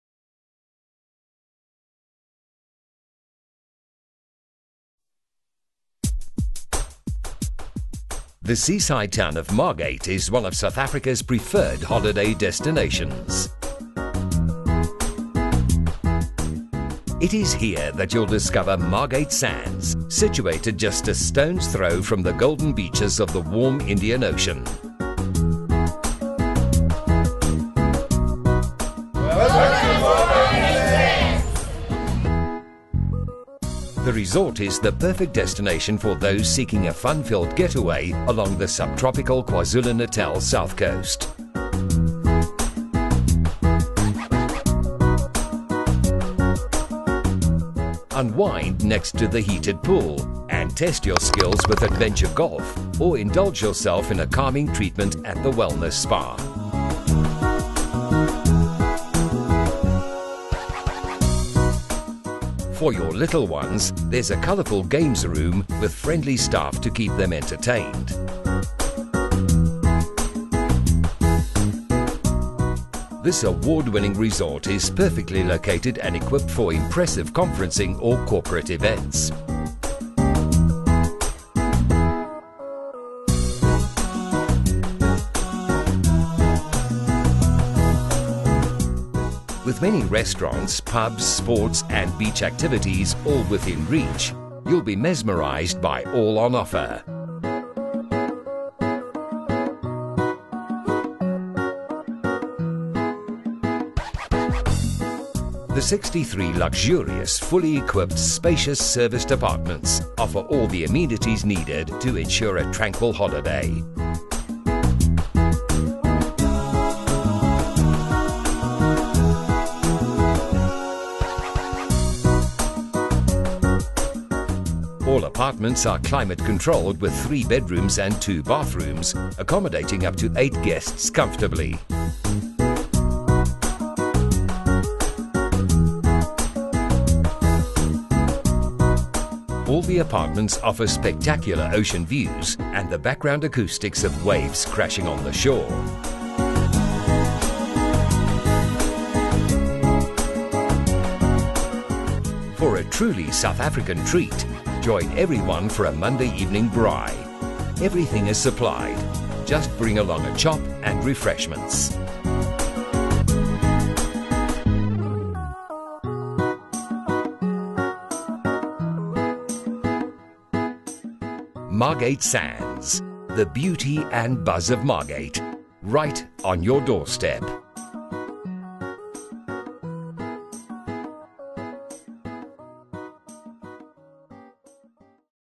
Margate Sands Beach Resort.
The seaside town of Margate is one of South Africa's favourite holiday destinations. It is here that you will discover Margate Sands, situated just a stones throw from the golden beaches of the Indian Ocean.  Margate Sands Resort is the perfect destination for those seeking a fun-filled getaway along the warm KwaZulu Natal South Coast.

Unwind next to the azure blue pool, have fun in the games room and test your skills with adventure golf or indulge yourself with a relaxing treatment at the Wellness Spa.

This award-winning resort is perfectly located and equipped for unforgettable conferencing or corporate functions, with many restaurants, pubs, sports and beach activities right on your doorstep.

The luxurious, fully equipped, spacious serviced apartments offer all the amenities needed to ensure a relaxing holiday. All apartments are air-conditioned with 3 bedrooms and 2 bathrooms, accommodating up to 8 guests comfortably.

All the apartments offer spectacular ocean views that will take you breath away.

Margate Sands, the beauty and buzz of Margate, right on your doorstep.